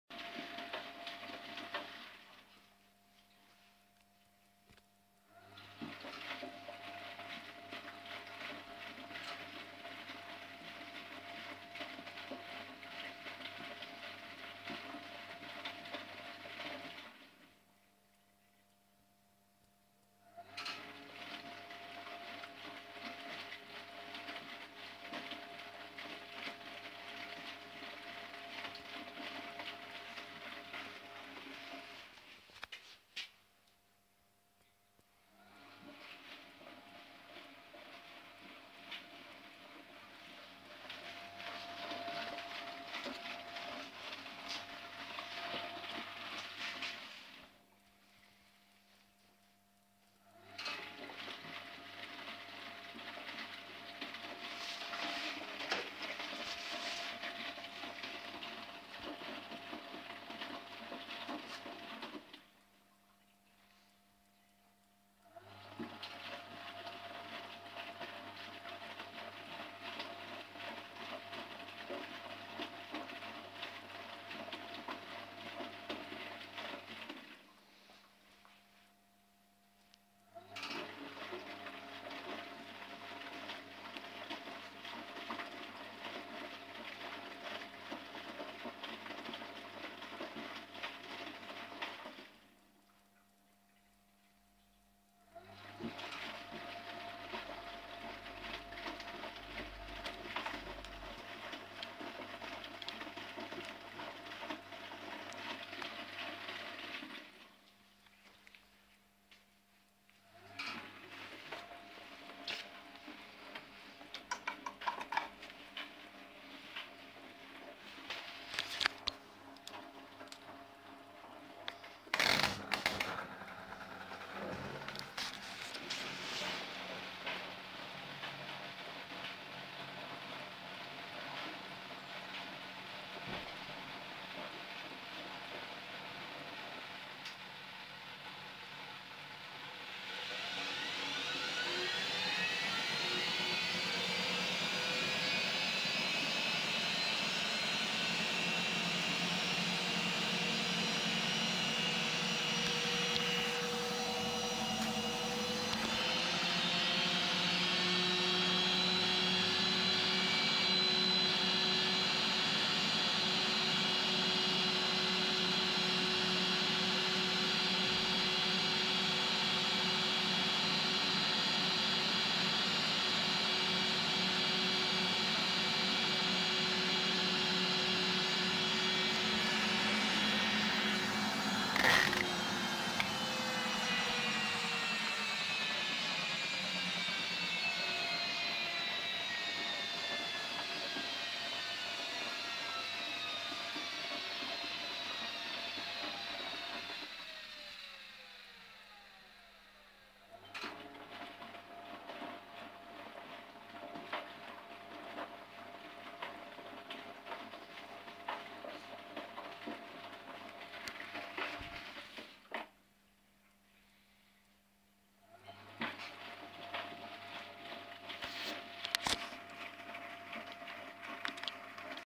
Asko 20005: Program 2 Eco 60
Woohoo, Wasmachineman is uploading another vid of his Askos for once!

Finally managed to do a wash in my 20005, better weather at last.

And yes, that is a Miele WT745. I couldn't get the heater working so I sold it.